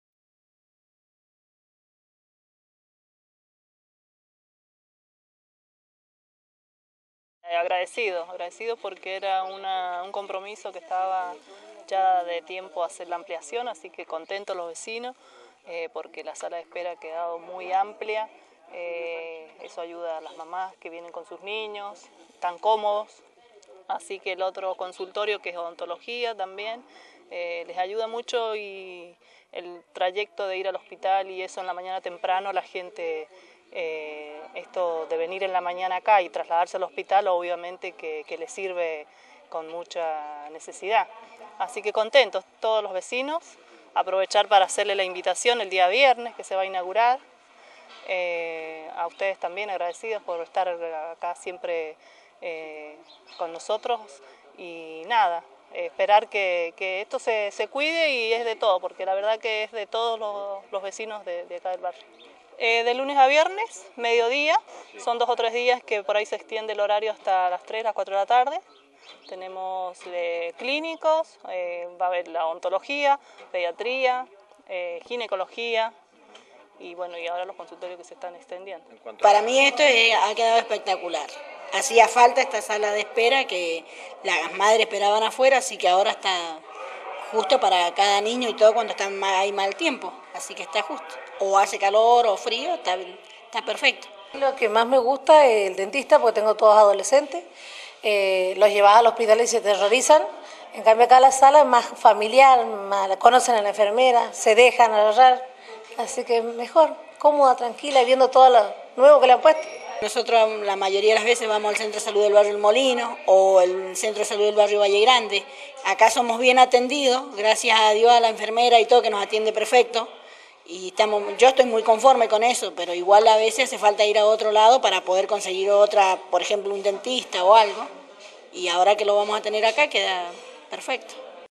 SALUDABLE ALEGRÍA
Entre sonrisas y emociones compartidas los vecinos y vecinas del Barrio Las Margaritas celebran la positiva respuesta del Municipio Sanrafaelino